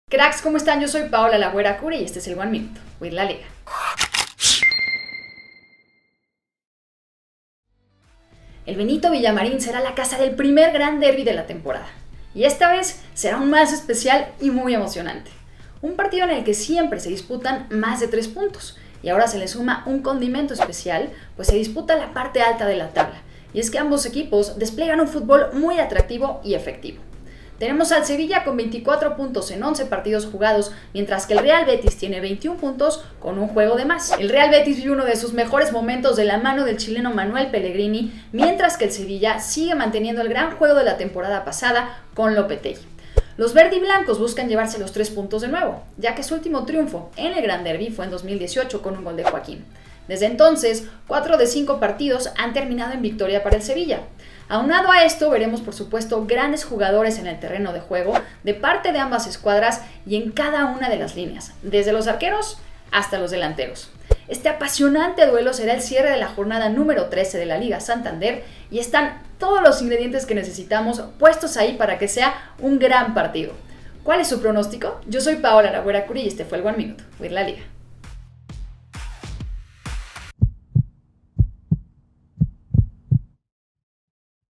One minute with LaLiga & ‘La Wera‘ Kuri: EL Gran Derbi
Se acerca ElGranDerbi, un duelo lleno de historia de pasión.
¿Quién agregará su nombre a la lista de gran historia
Disfruta este One Minute con LaLiga y Wera Kuri.